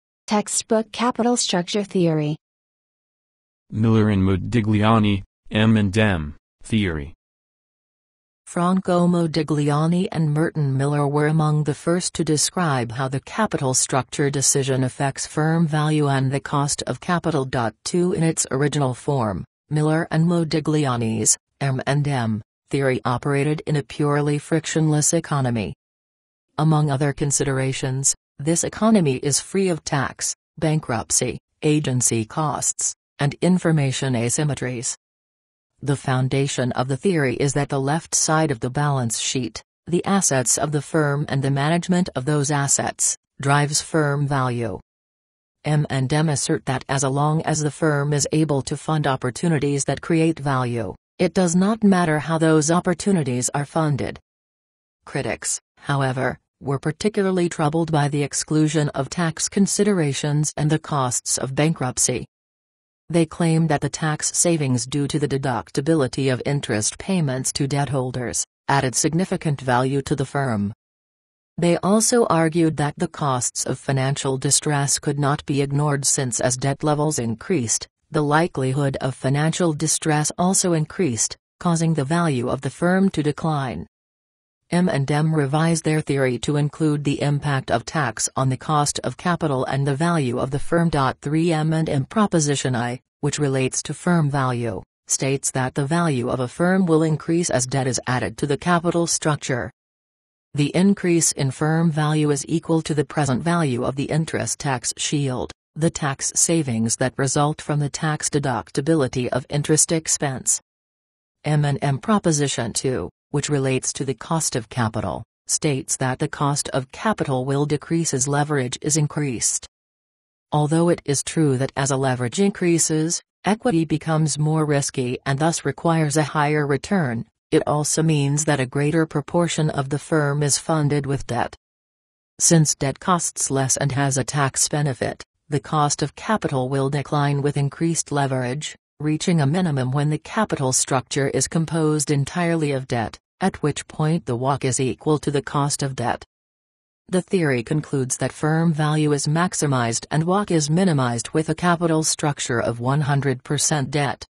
Miller and Modigliani (M&amp;M) Theory
TEXTBOOK CAPITAL STRUCTURE THEORY
Franco Modigliani and Merton Miller were among the first to describe how the capital structure decision affects firm value and the cost of capital.2 In its original form, Miller and Modigliani's (M&M) theory operated in a purely frictionless economy. Among other considerations, this economy is free of tax, bankruptcy, agency costs, and information asymmetries. The foundation of the theory is that the left side of the balance sheet—the assets of the firm and the management of those assets—drives firm value. M&M assert that as long as the firm is able to fund opportunities that create value, it does not matter how those opportunities are funded. Critics, however, were particularly troubled by the exclusion of tax considerations and the costs of bankruptcy. They claimed that the tax savings due to the deductibility of interest payments to debt holders, added significant value to the firm. They also argued that the costs of financial distress could not be ignored since as debt levels increased, the likelihood of financial distress also increased, causing the value of the firm to decline. M&M revised their theory to include the impact of tax on the cost of capital and the value of the firm.3 M&M proposition I, which relates to firm value, states that the value of a firm will increase as debt is added to the capital structure. The increase in firm value is equal to the present value of the interest tax shield, the tax savings that result from the tax deductibility of interest expense. M&M proposition II, which relates to the cost of capital, states that the cost of capital will decrease as leverage is increased. Although it is true that as leverage increases, equity becomes more risky and thus requires a higher return, it also means that a greater proportion of the firm is funded with debt. Since debt costs less and has a tax benefit, the cost of capital will decline with increased leverage, reaching a minimum when the capital structure is composed entirely of debt, at which point the WACC is equal to the cost of debt. The theory concludes that firm value is maximized and WACC is minimized with a capital structure of 100 percent debt. ...